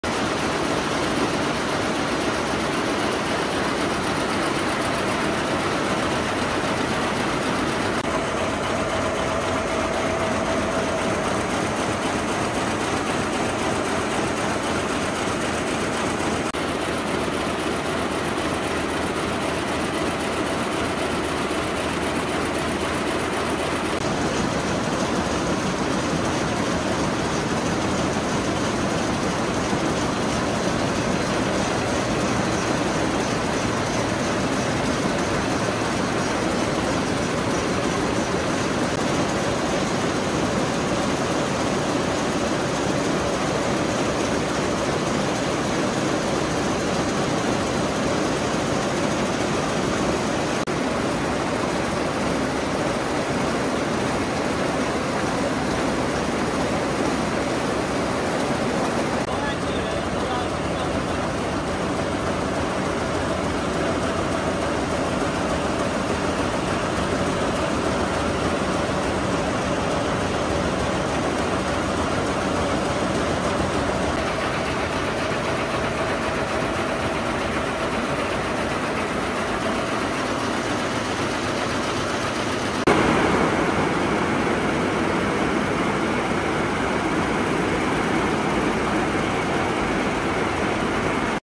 How To Make High-Quality Fabric Labels? Label Loom Production: Efficient And Precise Manufacturing !
Welcome inside our experienced facility! In this video, we take you through an in-depth exploration of the production process of a label loom. As an industry leader, we will share our expertise and best practices with you, demonstrating how to provide customers with high-quality fabric labels through an efficient and precise manufacturing process. From design to production, we will show you every step and share our years of accumulated experience and technology.